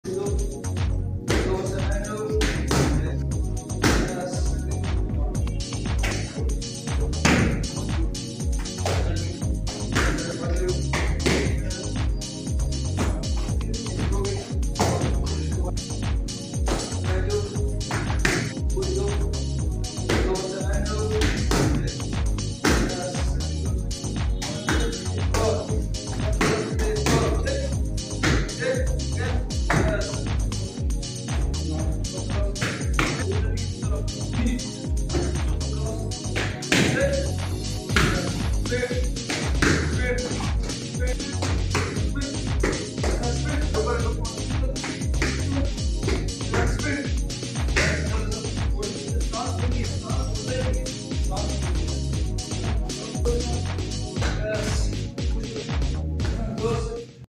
Here you can see the improvements of our summer camp warriors | Padwork | Future Champs | Team AJ
Head Coach Ali Ahmad Jan Hung Garian